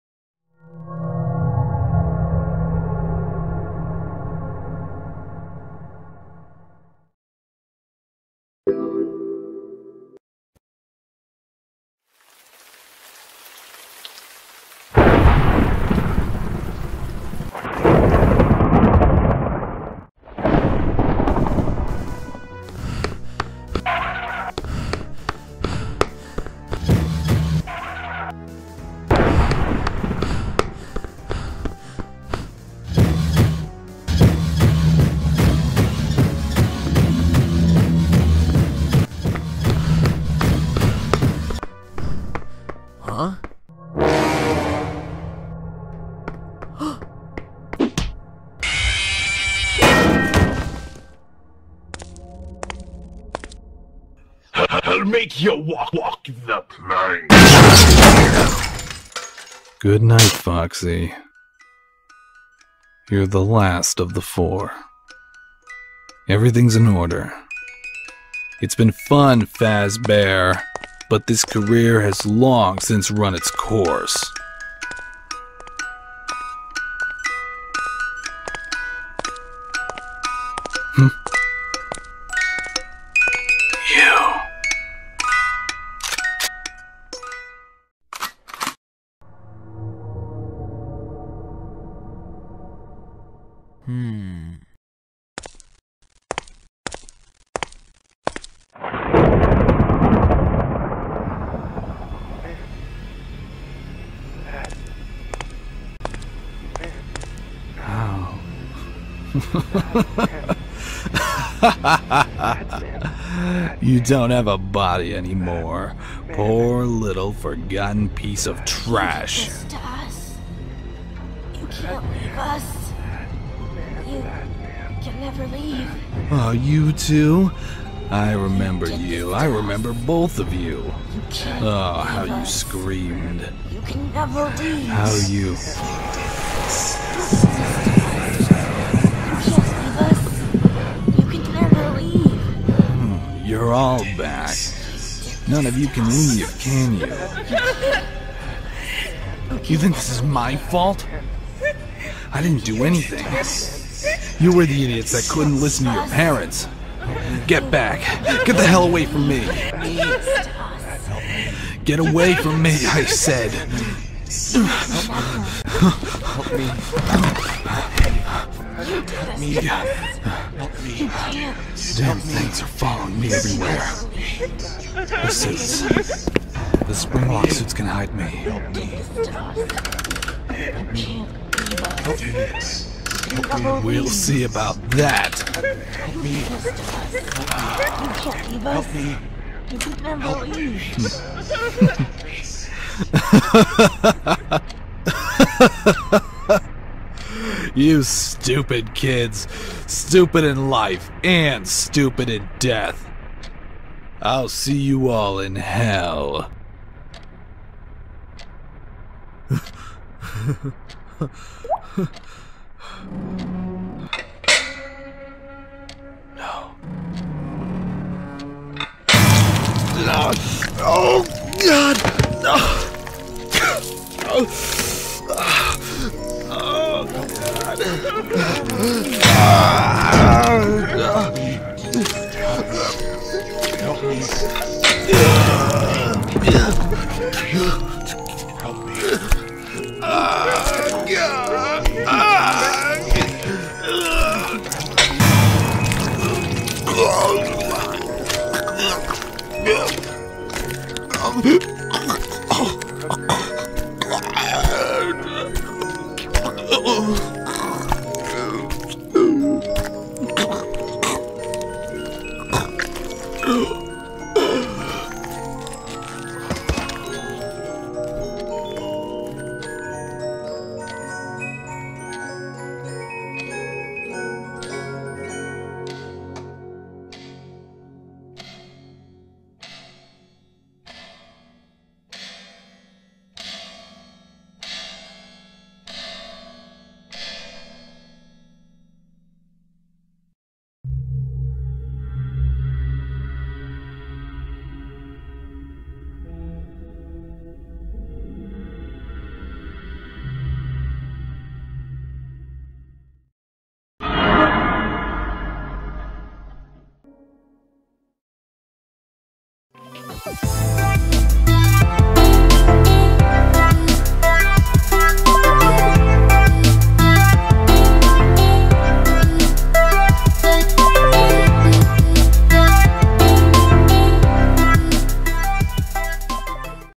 The Death of Purple Guy Remastered (Animation)
3 years ago, I created an animation of The Death of Purple Guy. For the longest time, I wanted the remake that animation since the animation hasn't aged well and there was no sound. So after lots of hard work, I remastered that old animation. Here is the final product, enjoy! :)

Also, I cut out the end credits because the music I used was copyrighted. 😕

CREDITS:

Written, Directed, and Animated by NQ Productions

Starring:

NQ Productions as Cassidy/Golden Freddy
MrCreepyPasta as Gabriel/Freddy
CreepyPastaJr as Jeremy/Bonnie
KittenReadsHorror as Susie/Chica
WellHeyProductions as Fritz/Foxy
MissShadowLovely as Charlie/Puppet
and Litterbot as William Afton

Original Story and Audio by MrCreepyPasta and Goldc01n

Inspiration From Zapper770

NQ Productions